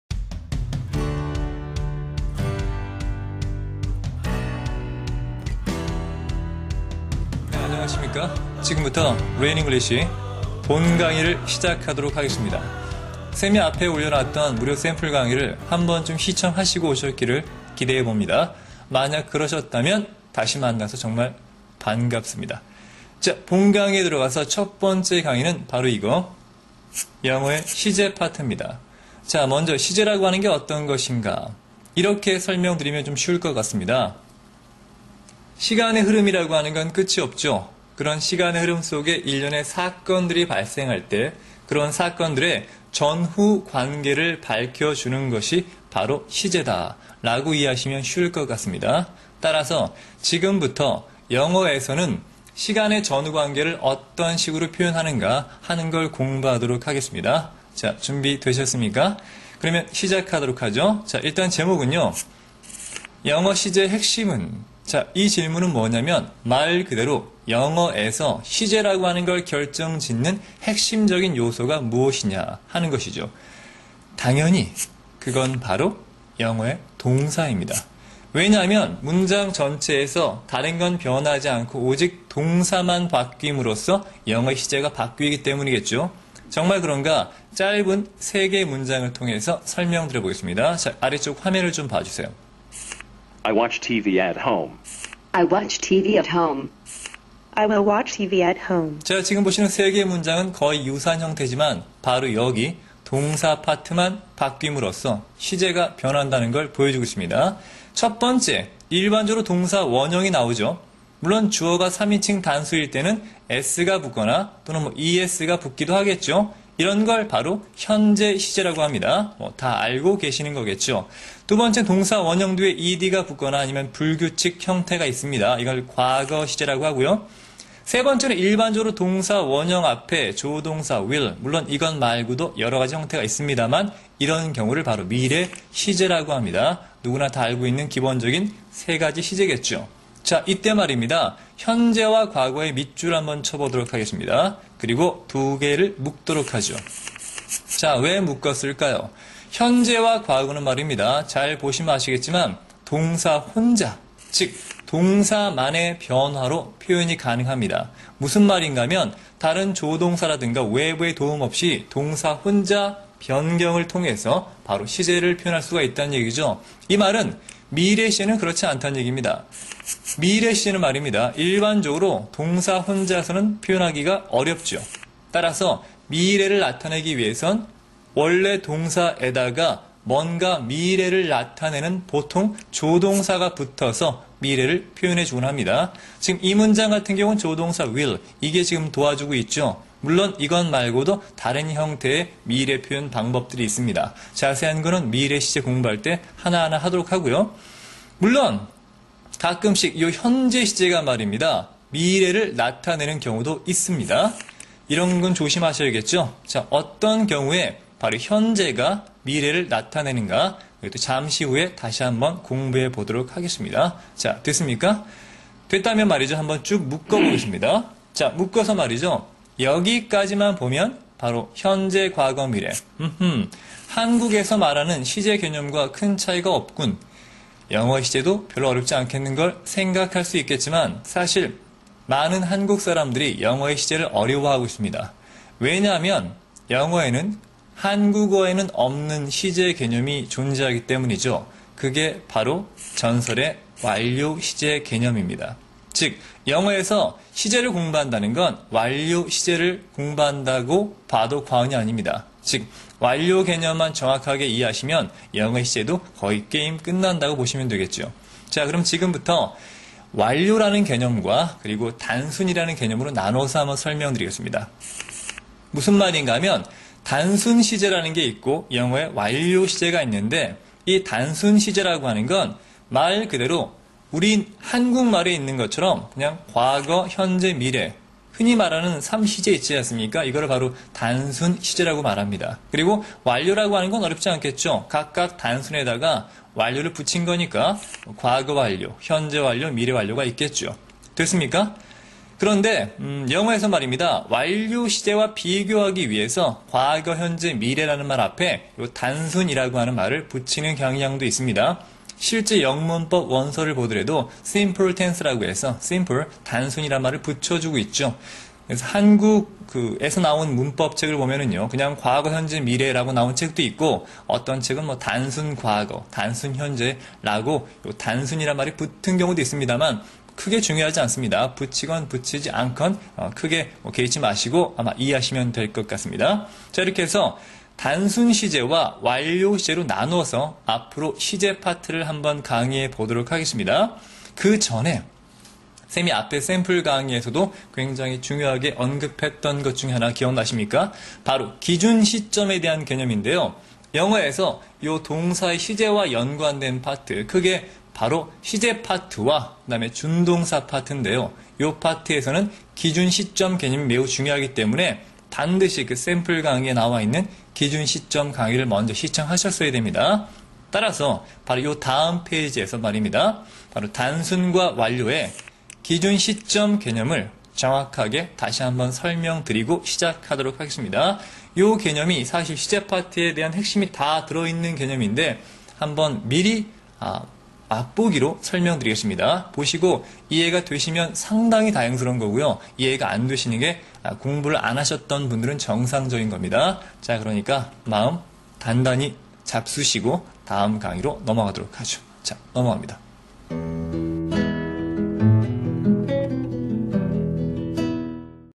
레인잉글리쉬 / 02.시제 [기초 개념] / 제01강 / 영어 시제의 시작
02. 영어의 시제 (The Tense) Part 1 : 단순 시제
레인잉글리쉬 / Rain English / 본강의 시작합니다.

[ 기초 개념 ]
01. 영어 시제의 시작
02. 기준 시점의 개념
03. 영어의 12시제?

[ 단순 시제 ]
04. 단순시제 기본 개념
05. 단순과거 (시점과 기간의 차이)
06. 단순현재 기본개념
07. 현재시제 유의사항 1 (시간부사절)
08. 현재시제 유의사항 2 (조건부사절)
09. 시간부사절이 아니라, 명사절 & 형용사절
10. 조건부사절이 아니라, 명사절

[ 단순 미래 ]
11. 미래시제 기본개념
12. Will & Shall
13. Be going to
14. Will / Be going to 차이점
15. 현재시제가 미래를?
16. 현재진행도 미래를?
17. Be about to (왜 가까운 미래지?)
18. Be on the point of ~ ing (~하기 직전?)
19. Be to 구문? (그냥 형용사 개념으로!)
20. Be supposed to (~하기로 되어있다)

[ 총정리 ]
21. 단순시제 총정리

03. 영어의 시제 (The Tense) Part 2 : 완료시제로 이어집니다.